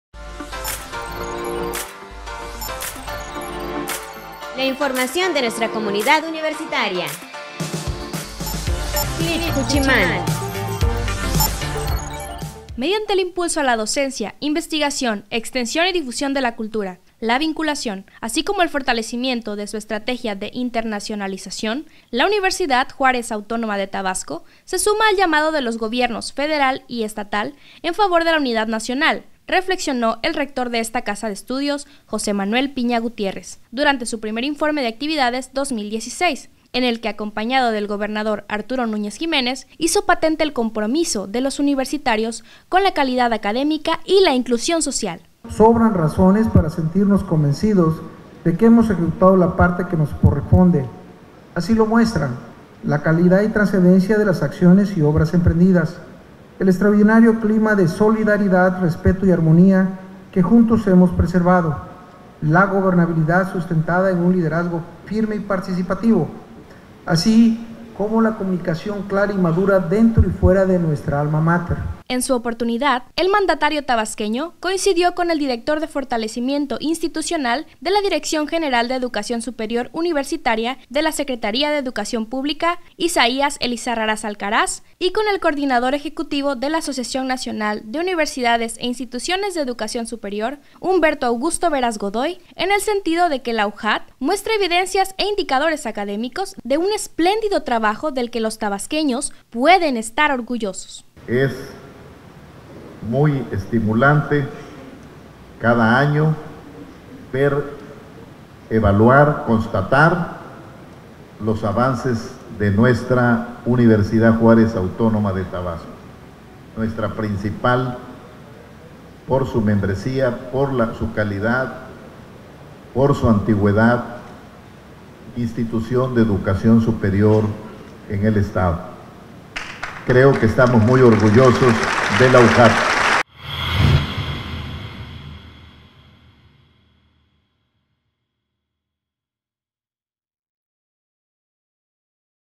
Presenta Rector, informe de Actividades
El Dr. José Manuel Piña Gutiérrez, presentó su Primer Informe de Actividades, que constata los avances en materia de docencia, investigación, vinculación, gestión moderna e internacionalización